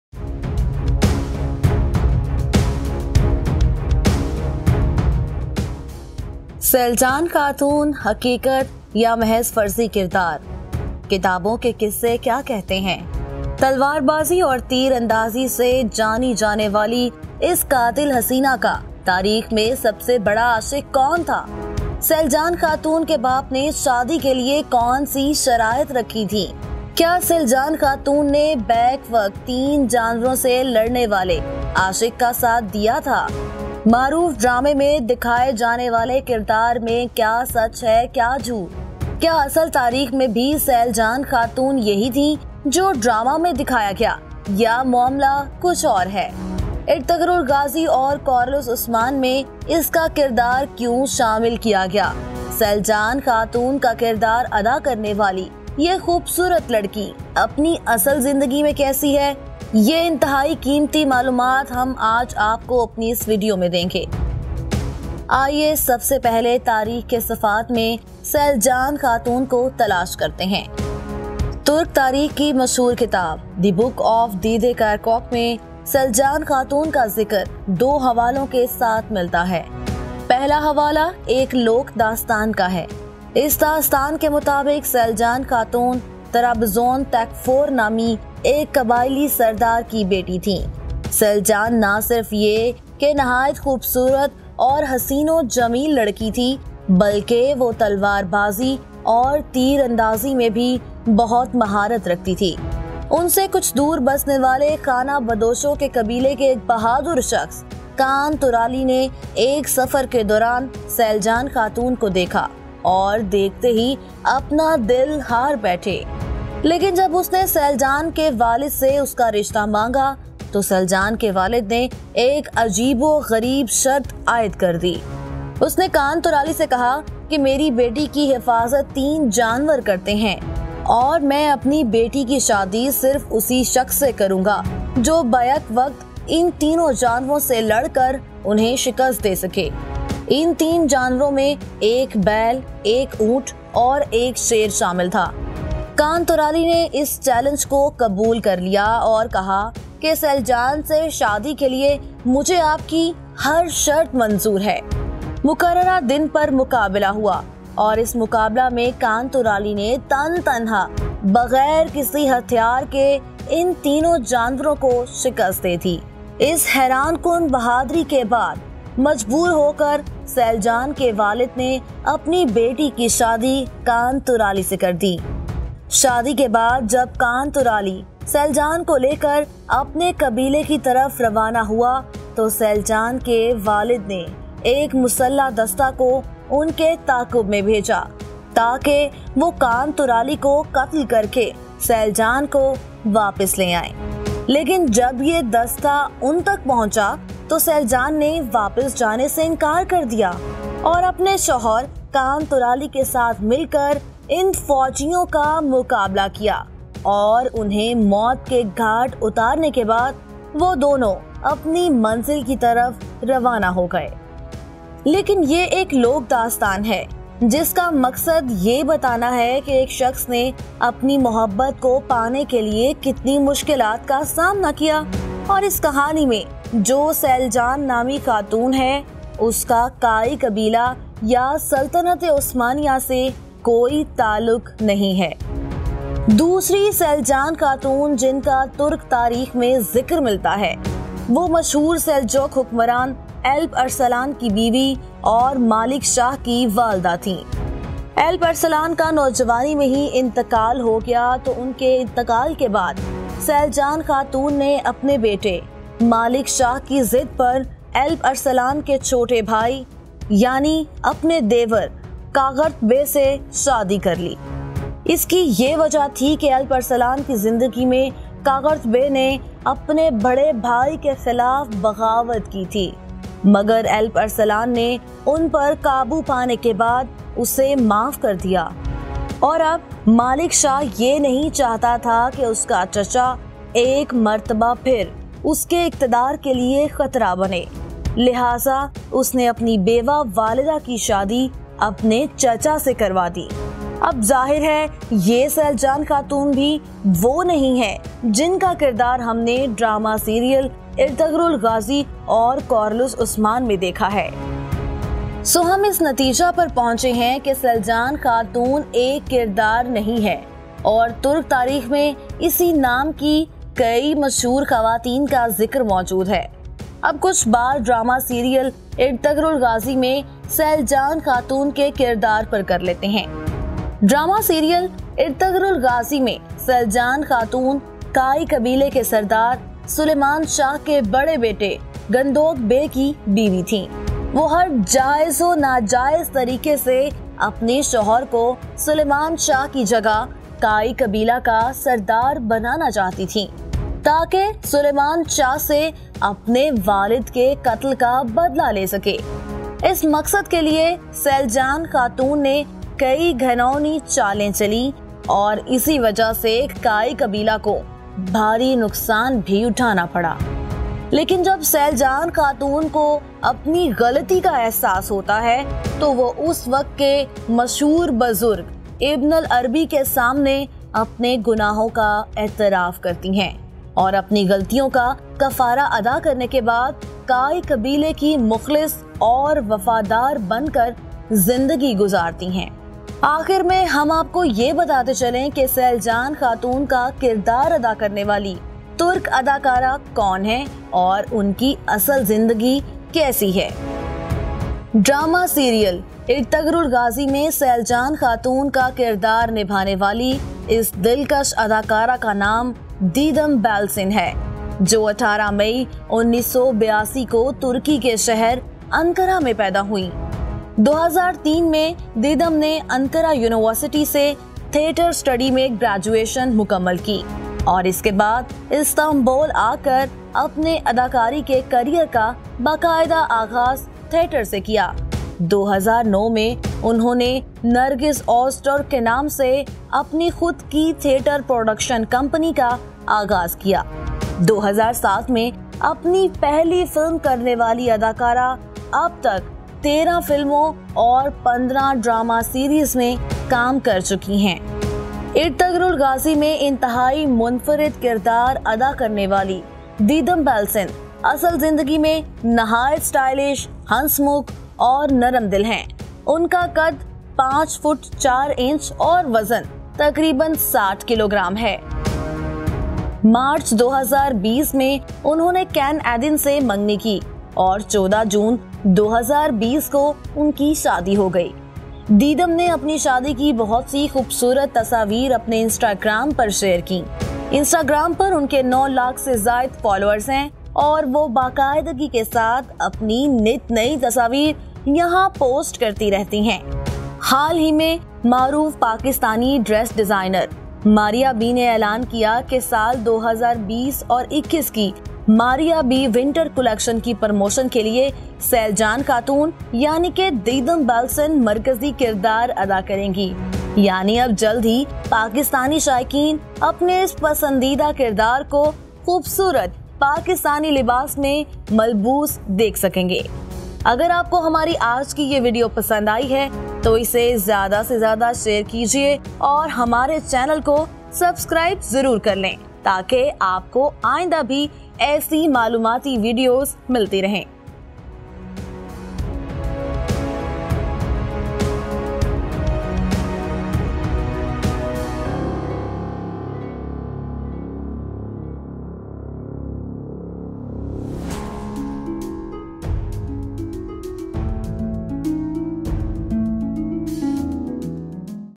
Real History of Selcan Hatun in Kurulus Osman Season 2 | Nuktaa
Why Selcan Hatun is back in Kurulus Osman Season 2?
In Ertugrul Ghazi, Selcan Hatun was wife of Gündoğdu Bey and daughter-in-law of Hayme Hatun and Suleyman Shah. She was older sister of Gökçe. When Selcan realised the great sin she had been doing, she seek audience of Ibn Arabi. She was advised to do Tawbah Nasuha and after that she became a loyal member of Kayi Tribe.